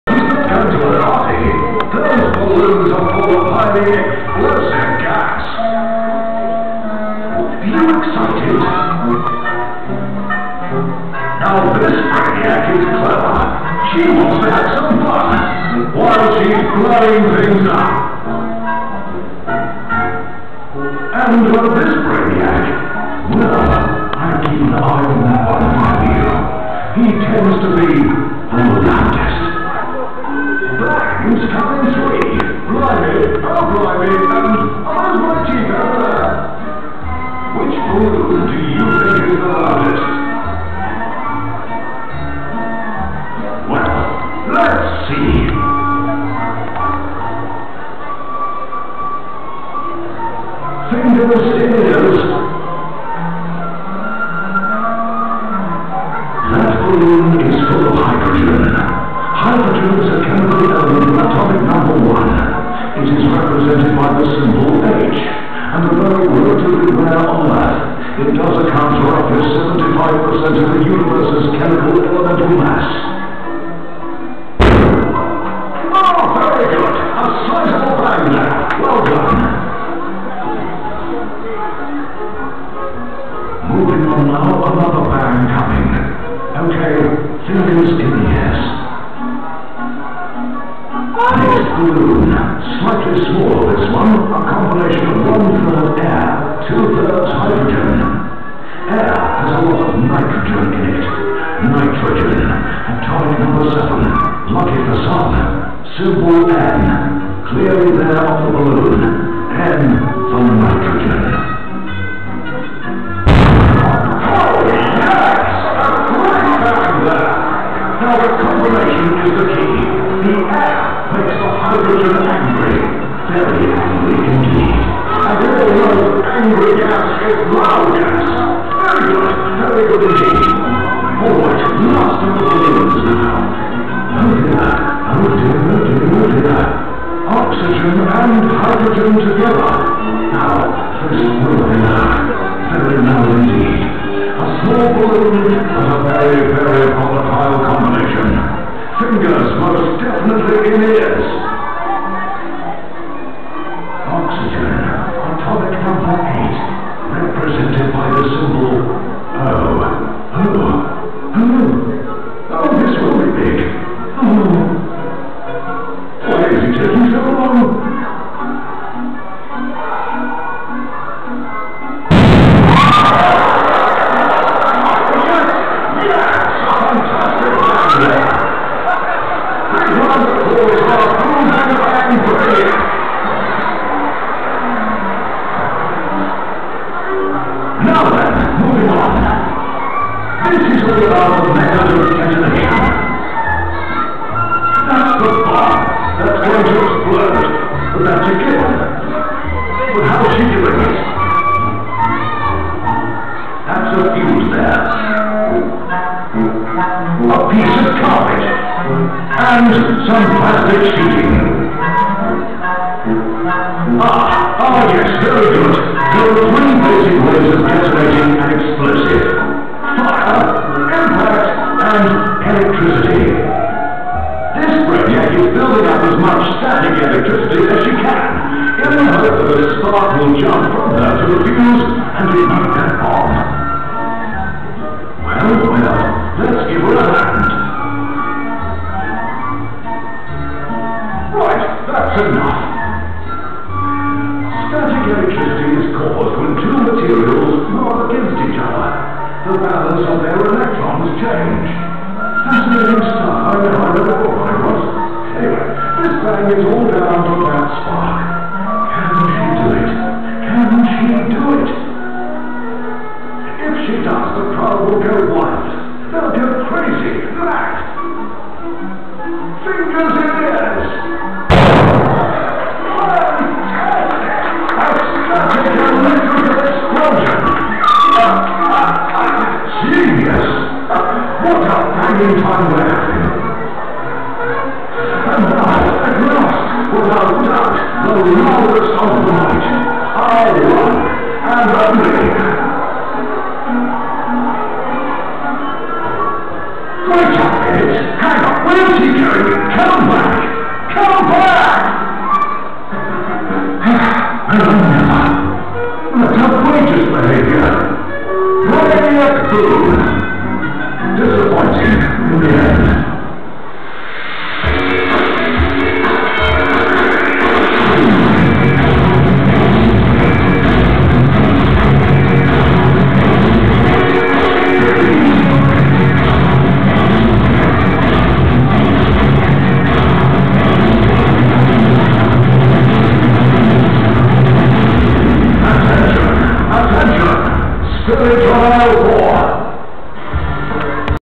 Butlins Skegness - Braniac Live - Big Bangs
Braniac live at Butlins Skegness during my 2011 holiday.